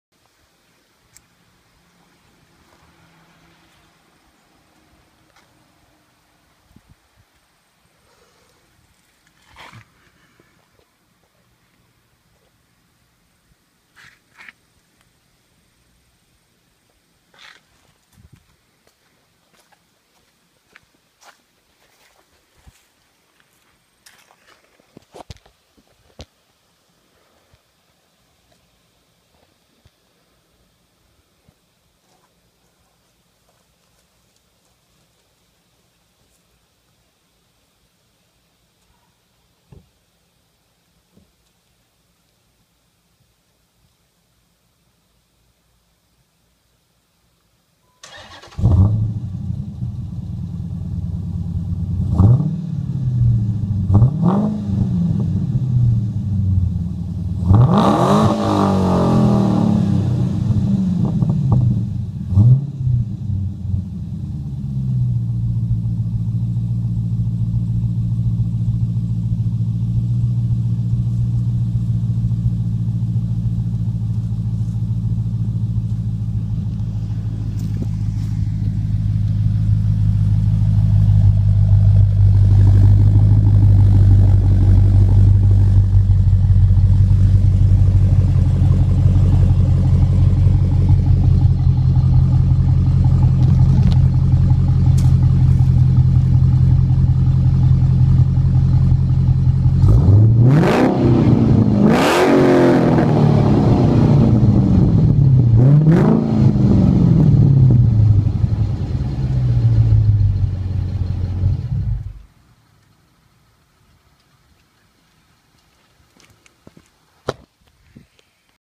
2012 Dodge Challenger SRT8 with 12'' Glasspack CherryBombs
My challenger with glasspack that I just installed today I'll make another video in the future when the fiberglass is all burned out